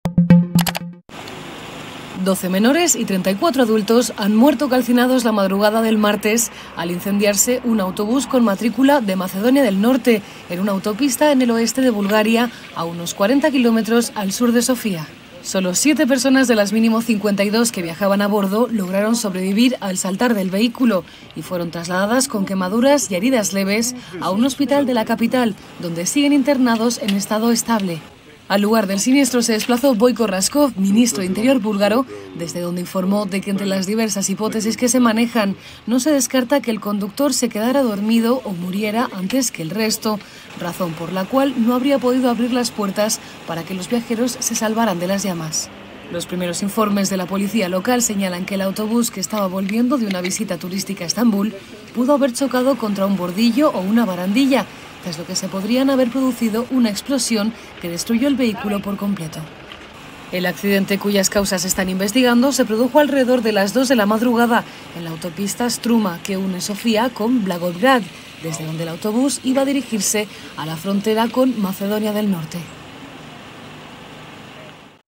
Mueren quemadas 46 personas en un bus, 12 eran menores en Bulgaria
Un total de 12 menores y 34 adultos murieron esta madrugada al incendiarse un autobús en una autopista en el oeste de Bulgaria, cerca de la ciudad de Bosnek, a unos 40 kilómetros al sur de Sofía, informó el ministerio del Interior búlgaro